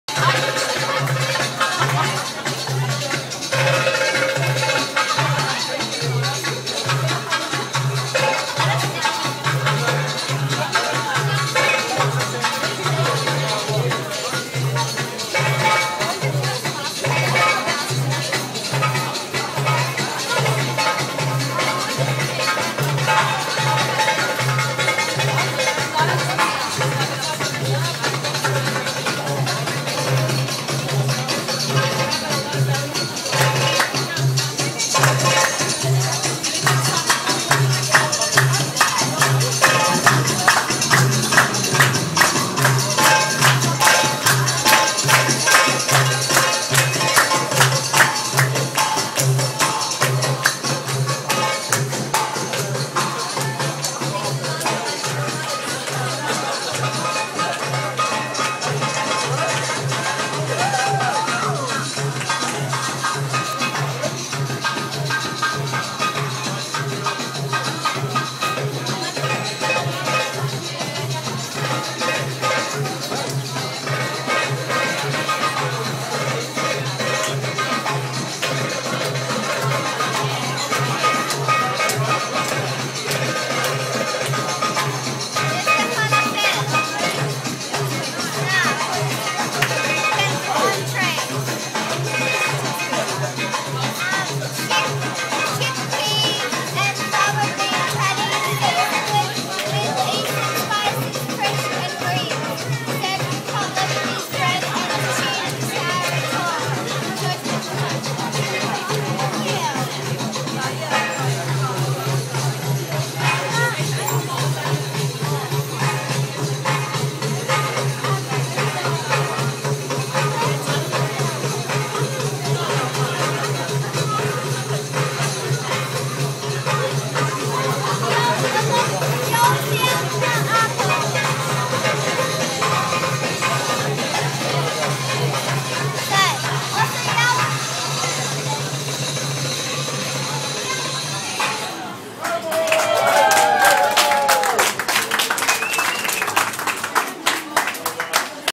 Belly Dancer Princess Jasmina of Melbourne
Live fully improvised belly dancing by  Melbourne's superstar favourite Princess Jasmina! Totally unpremeditated, non-choreographed,  non-routined belly dance.